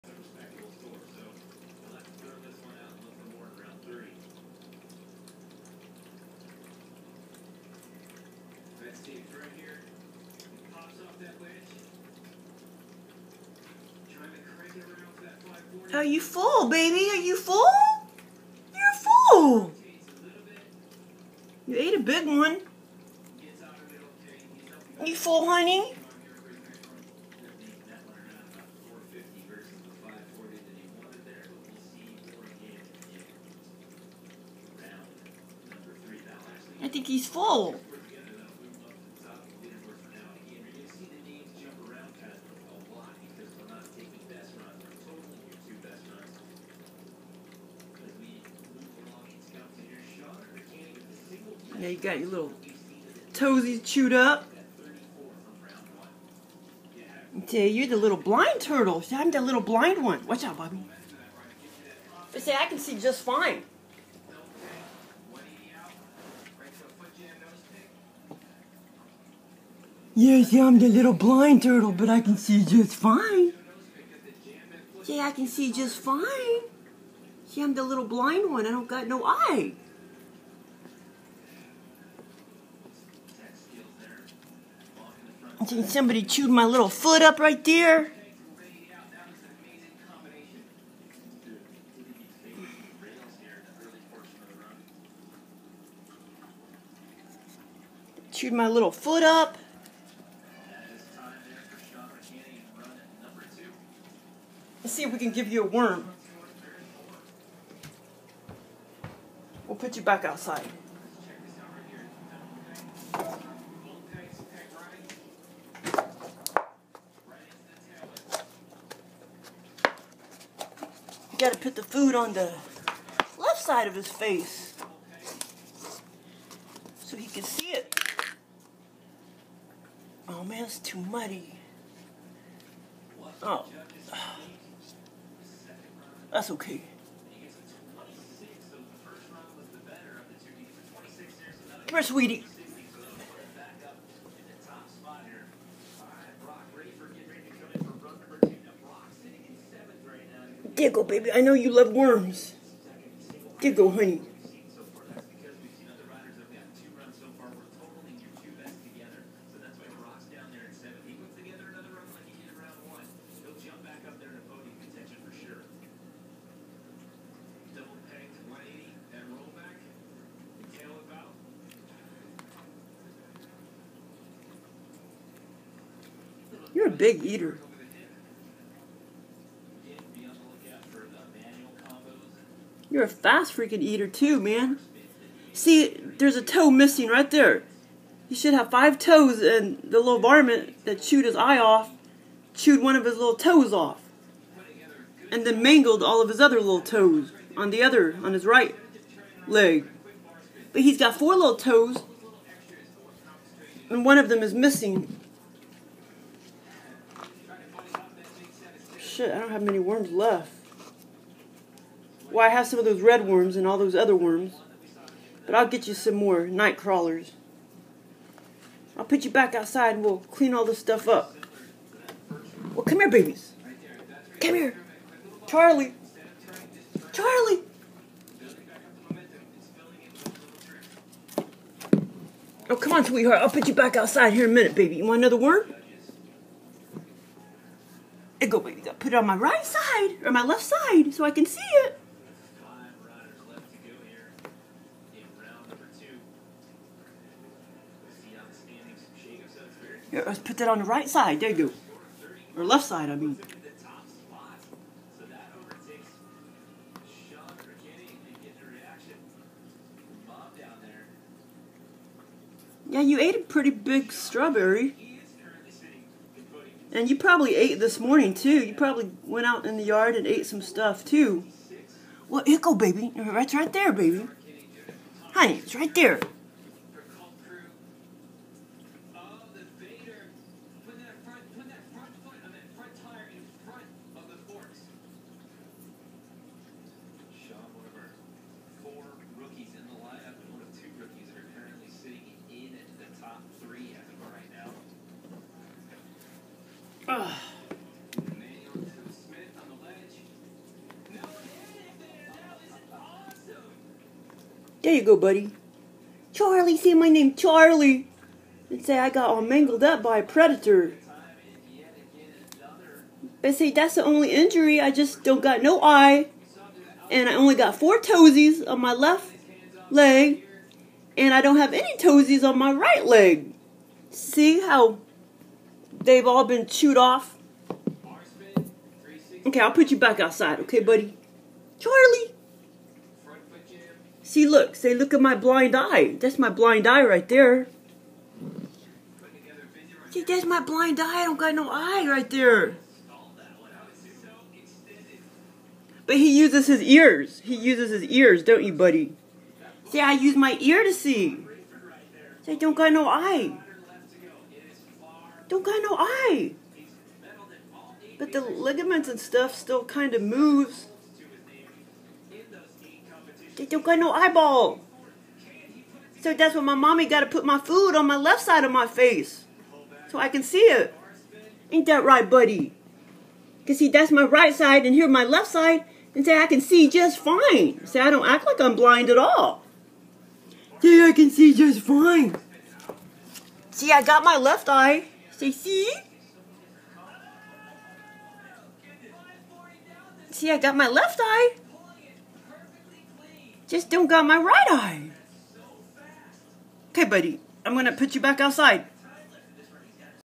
One Eyed blind Turtle (3toed box turtle)
His name is Charlie. A co worker gave him to me.
1 of his left toes have been eaten off. He should have 5 but he has  four.   And the predator ate his right eye.  AND chewed on all toes on his right foot. HE is fine and doesn't   act blind.  He navigates the yard just fine.  He is only blind in one eye. It lookes like the whole eye is gone or closed up. I still see tendons moving in that blind eye. He loves strawberries and worms.   I wonder if a big field rat  nipped on him.  They have 3 long toes in the back feet. None have been nipped off. That is why this species is called 3toed box turtles.

Females have brown eyes and males  RED/ORANGE.